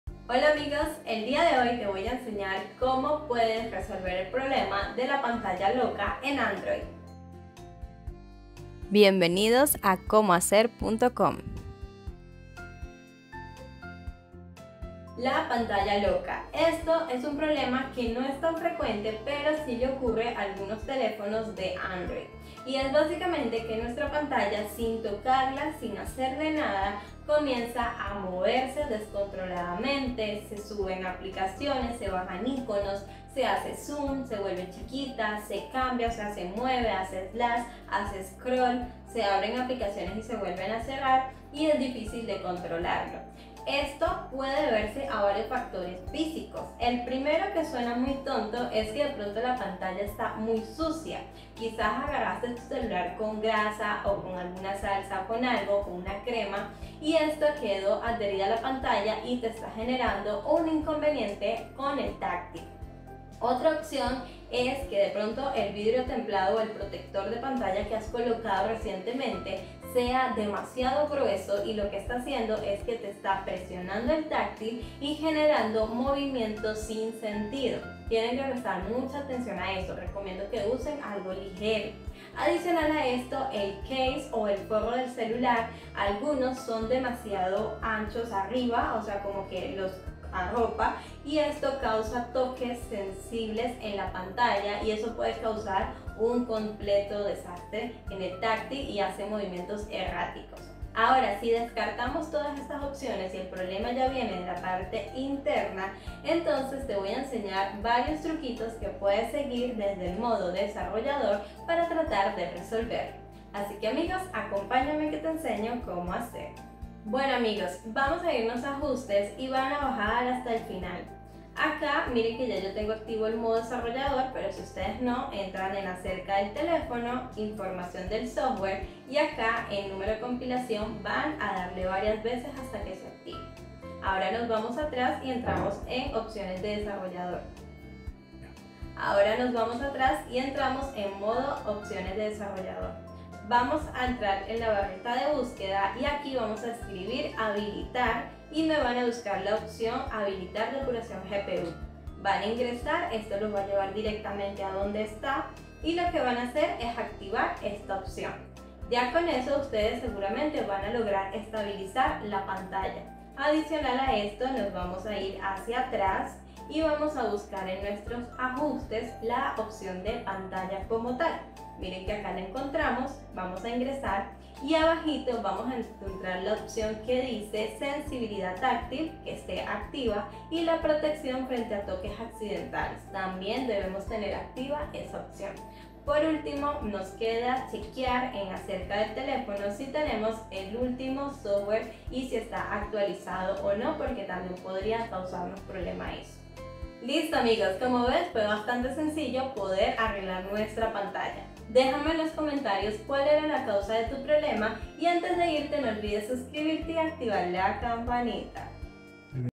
Cómo Arreglar la PANTALLA LOCA en Android | 😵​ Se Mueve sola ​😵
¡Saludos a todos los usuarios de Android! En este video, abordaremos el molesto problema de la PANTALLA LOCA en dispositivos Android. Si has experimentado situaciones en las que la pantalla responde de manera impredecible o muestra comportamientos extraños, ¡has llegado al lugar correcto! Te proporcionaremos soluciones rápidas y efectivas para recuperar la funcionalidad de tu pantalla en poco tiempo.